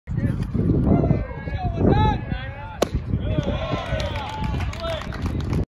Jon Shields, ambidextrous pitcher, 2023 against St. Pius Houston
Jon Shields, ambidextrous pitcher, 2023 against St. Pius Houston lefty v. Righty